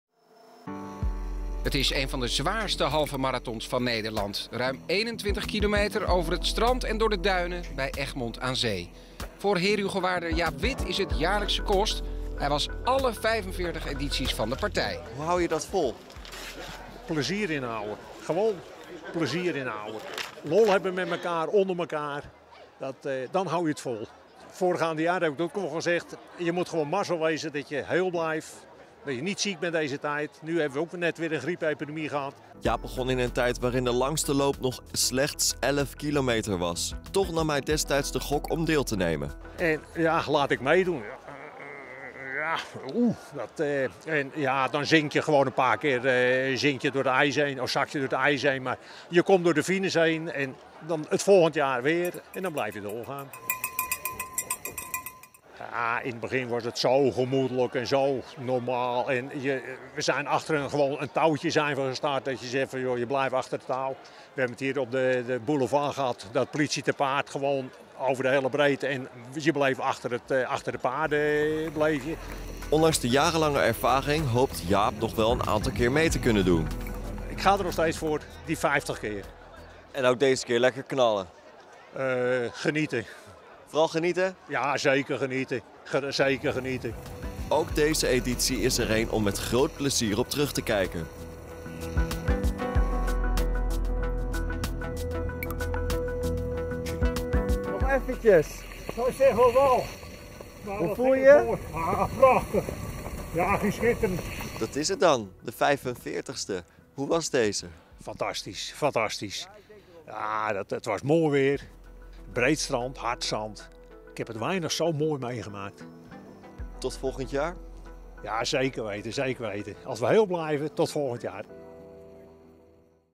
Jaap Wit: "Ik ga voor de vijftig keer"
Jaap Wit over de halve marathon van Egmond: "Ik ga voor de vijftig keer"